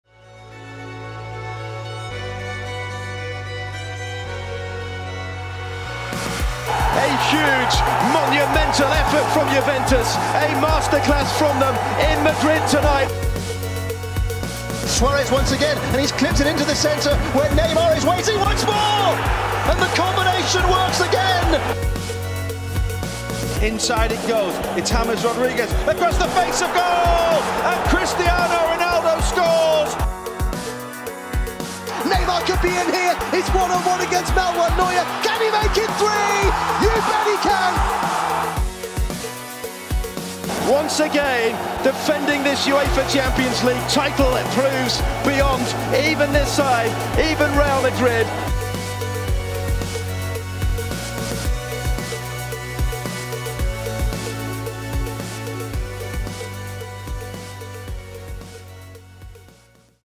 UEFA Champions League semi-finals in numbers
Neymar, Cristiano Ronaldo and Juventus all feature in our review of the facts and figures of the UEFA Champions League semi-finals.